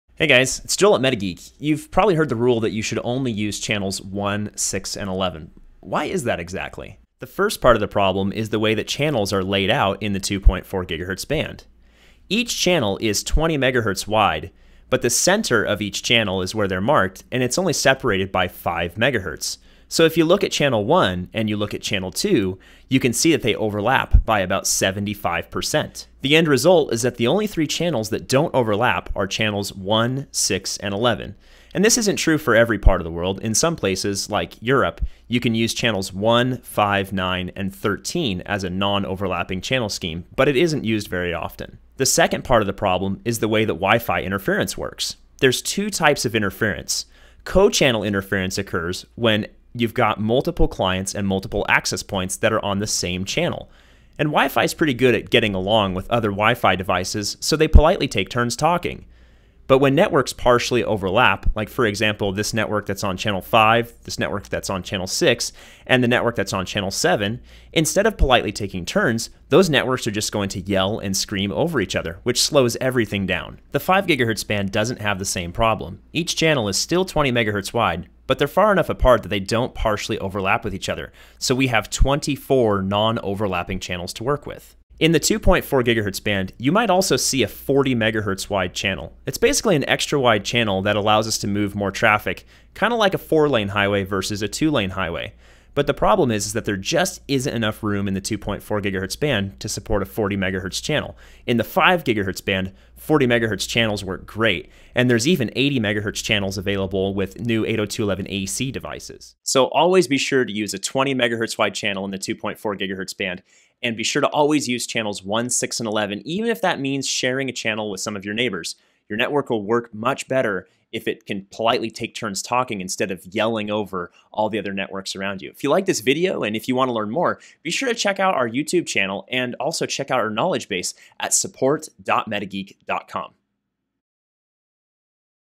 Which WiFi Channel Should I Pick? 1, 6, or 11: A MetaGeek Tutorial
WiFi channels can be difficult to pick. With 11 channels to choose from in the United States, and networks all over the place, it's hard to know where to set up a new network. Or is it?

In the 2.4 GHz band, 1, 6, and 11 are the only non-overlapping channels. Selecting one or more of these channels is an important part of setting up your network correctly. Currently, many wireless routers automatically select the channel for you upon initial setup, where depending on your wireless environment, it could lead to slow WiFi speeds and interference. This video will describe what interference you're dealing with and takes you through the steps to selecting the right channel, so you can understand why you should choose between channel 1, 6, and 11.

Are you on the right channel? Download MetaGeek's inSSIDer Lite now to find out!